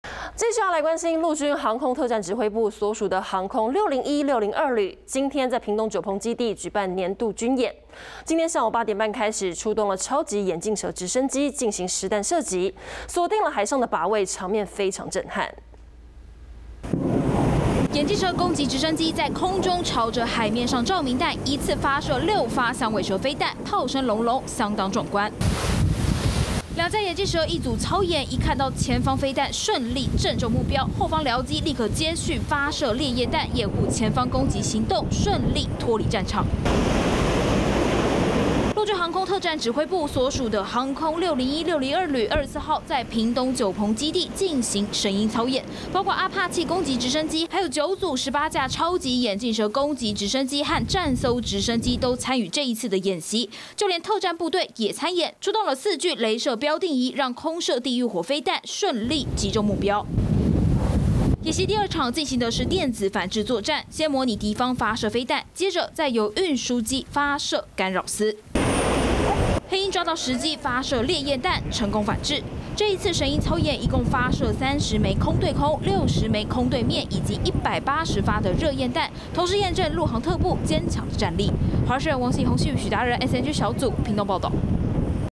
黑鷹.阿帕契亮相! 陸軍神鷹操演超震撼｜華視新聞 20210826
陸軍航空特戰指揮部所屬的航空601、602旅，在屏東九鵬基地進行年度的「神鷹操演」，今(26)日上午八點半開始，出動超級眼鏡蛇直升機，進行實彈射擊，鎖定海上靶位，場面相當震撼，還有黑鷹直升機，進行電子反制作戰，相當壯觀。